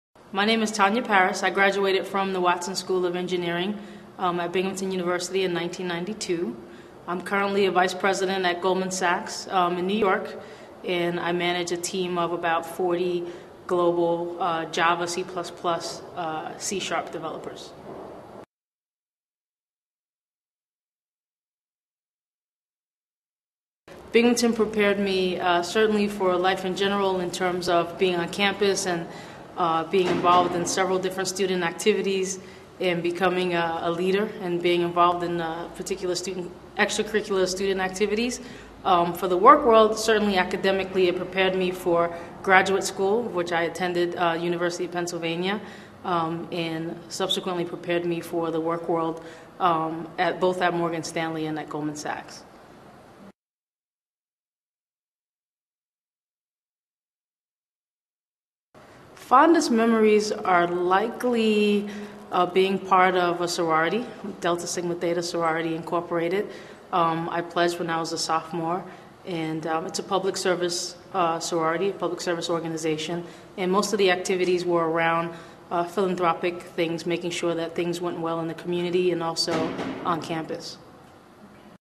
Think Binghamton - Tonya Parris '92
Transcript:
My name is Tonya Parris. I graduated from the Watson School of Engineering at Binghamton University in 1992. I am currently vice-president of Goldman Sachs at New York and I manage a team of about 40 global Java, C++ and C# developers.

Binghamton prepared me certainly for life in general in terms of being on campus and being involved in several student activities and becoming a leader and being involved in particular student activities. For the work level, it academically prepared me for graduate school, which I attended University of Pennsylvania and subsequently prepared me for the work world both at Morgan Stanley and Goldman Sachs.

Fondest memories are likely being part of a sorority, Delta Sigma Delta Sorority Incorporated. I pledged when I was a sophomore. It's a public service sorority, public service organization, and most of the activities were around philanthropic things, making sure that things worked well in the community and also on campus.